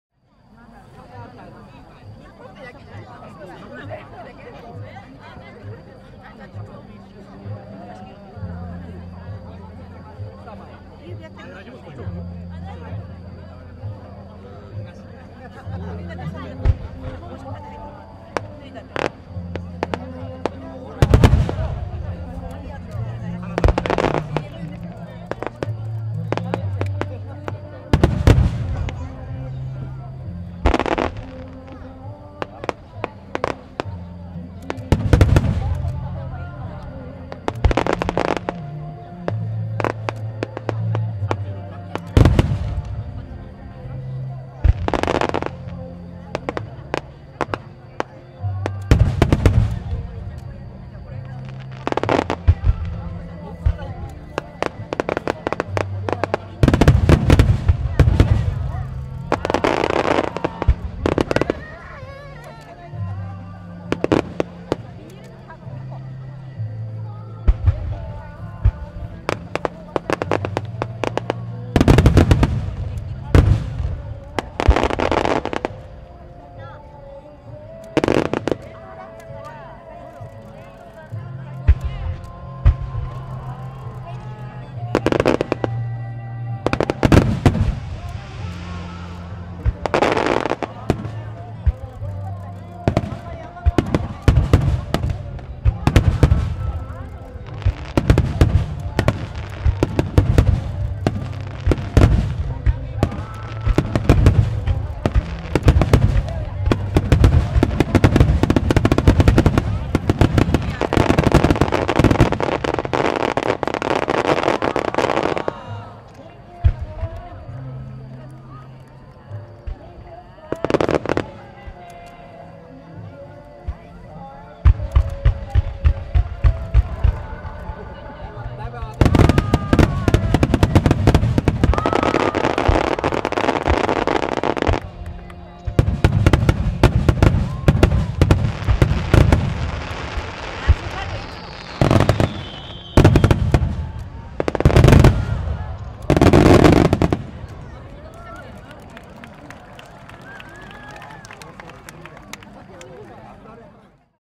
2012 土浦の花火 優秀賞 「彼方の光」 新潟煙火工業
第81回　土浦全国花火競技大会

新潟 新潟煙火工業(株)

スターマインの部　No.36
優秀賞
彼方の光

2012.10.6
81th TSUCHIURA ALL JAPAN FIREWORKS COMPETITION

Niigata Enka Industries Co.,Ltd.

STARMINE
Distant light